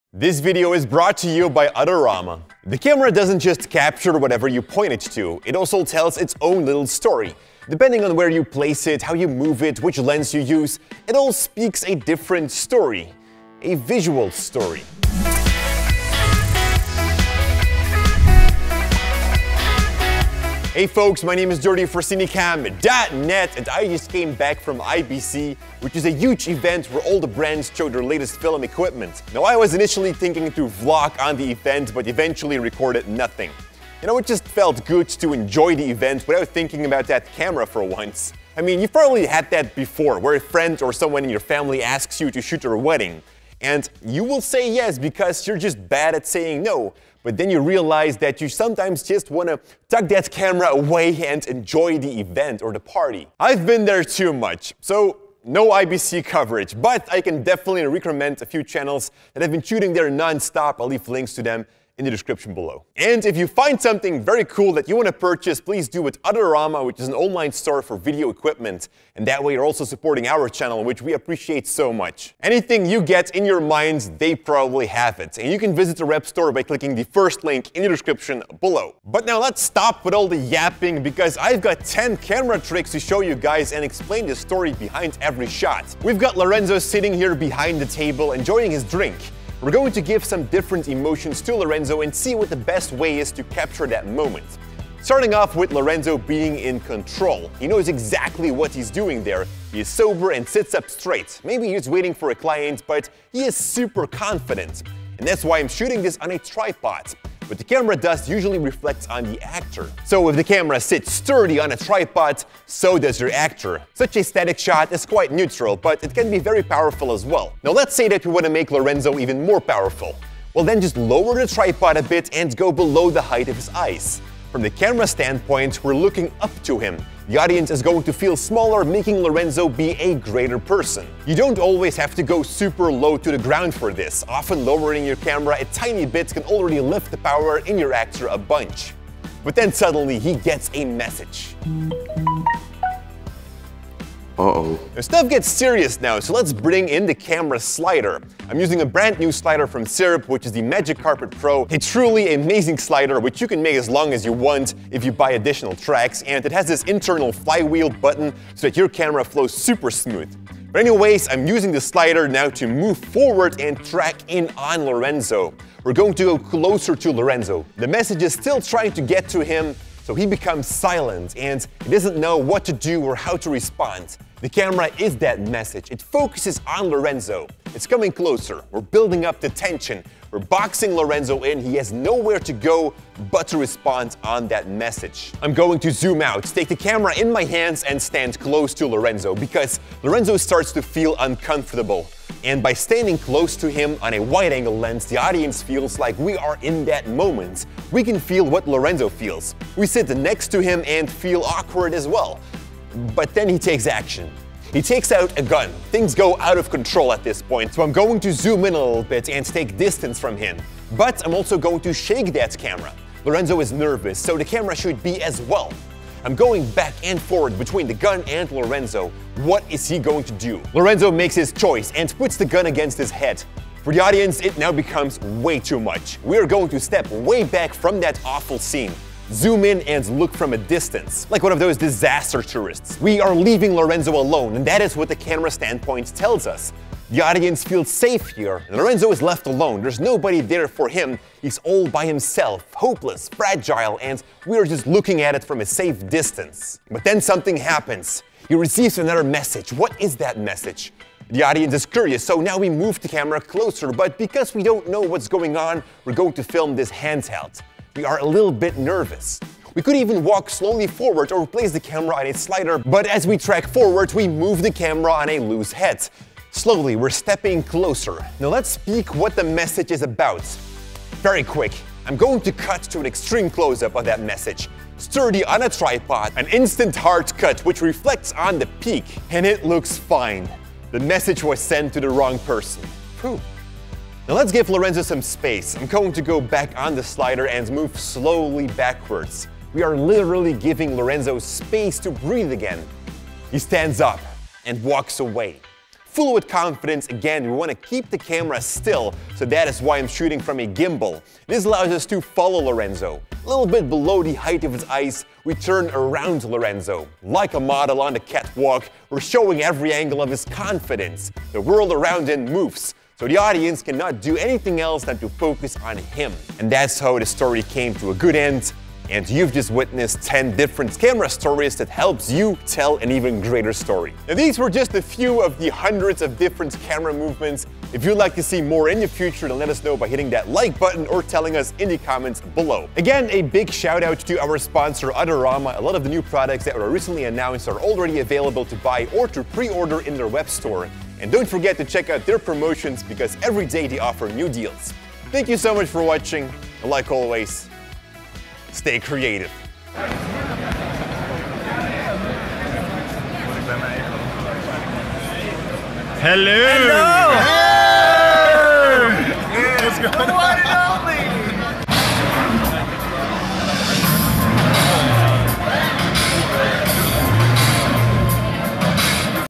MY CAMERA TOLD ME A STORY
🎬 Master Filmmaking, Video Editing, & VFX in One Bundle! - 10 camera tricks for telling a visual story. Learn how to move your camera, chose the right lenses and creating that perfect frame. Every possibility tells a different cinematography story.